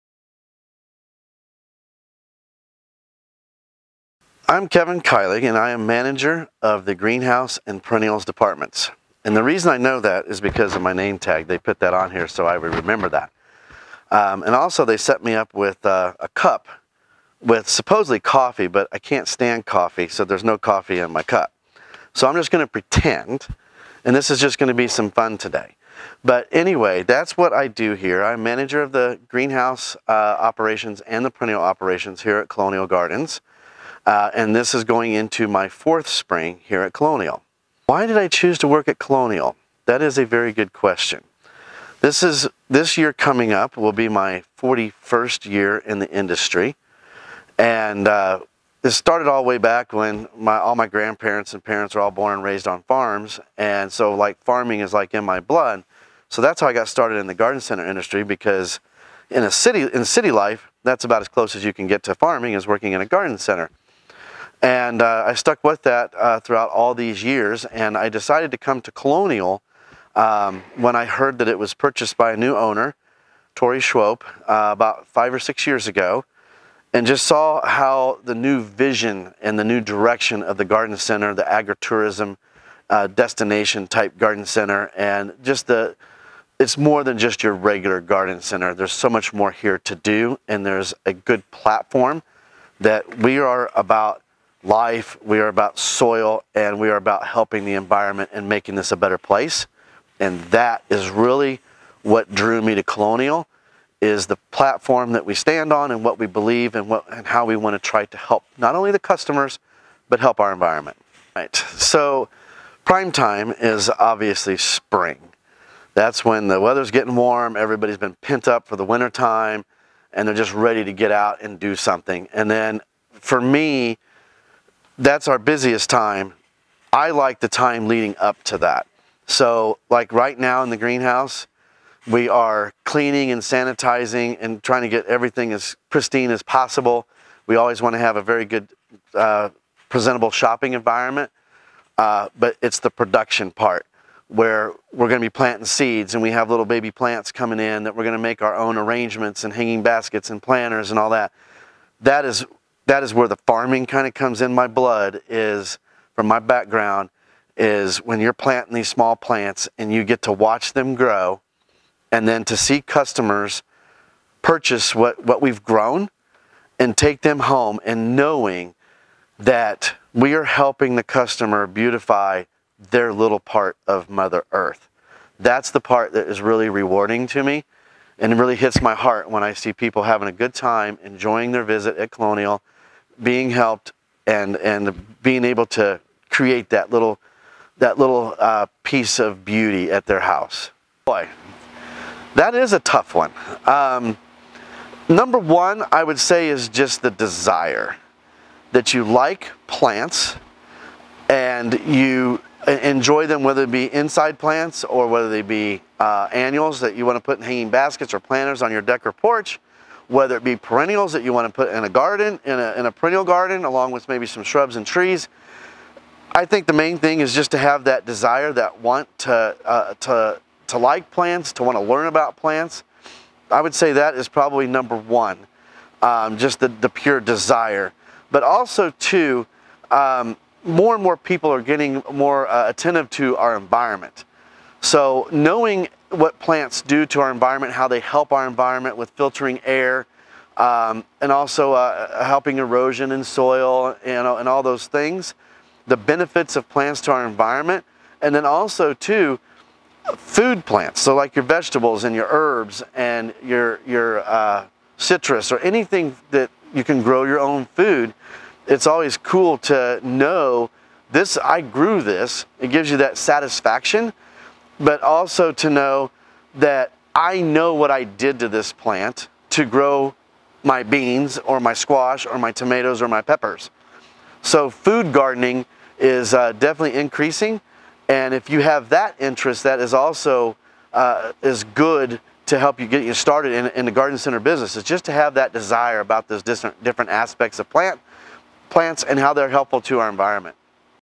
Coffee with Colonial - Kevin, Greenhouse Manager
Coffee with Colonial - Meet the Team series
Each week we will introduce a team member here at Colonial Gardens and answer questions about their job, department, history in the industry!